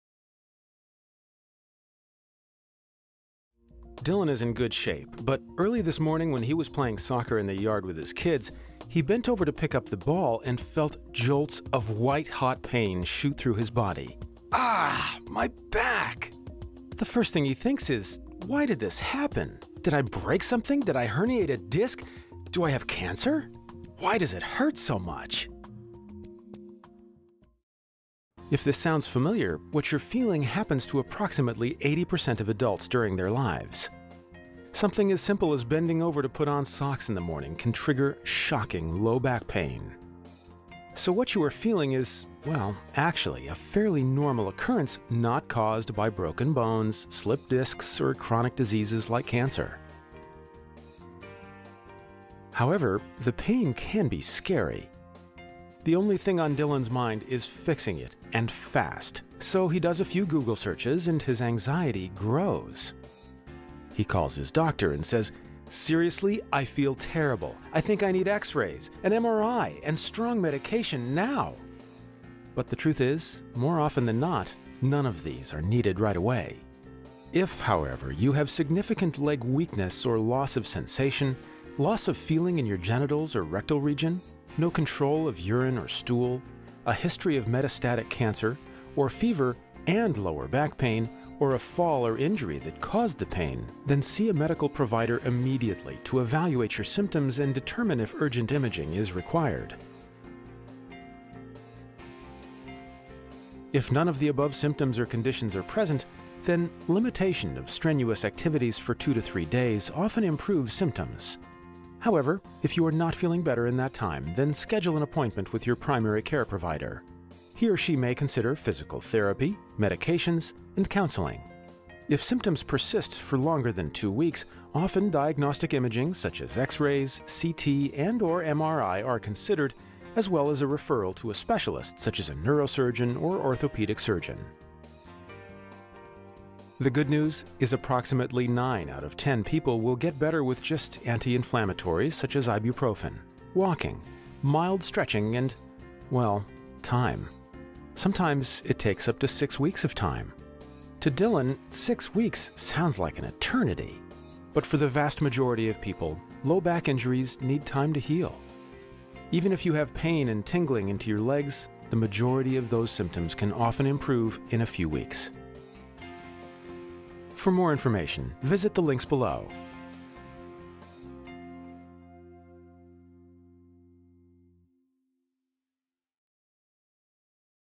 Low Back Pain: Basic Information and Steps Toward Healing
Sudden shocking back pain – it’s likely discomfort that will go away with time and care. Here are steps you can take to heal and symptoms to look for that may indicate an underlying issue.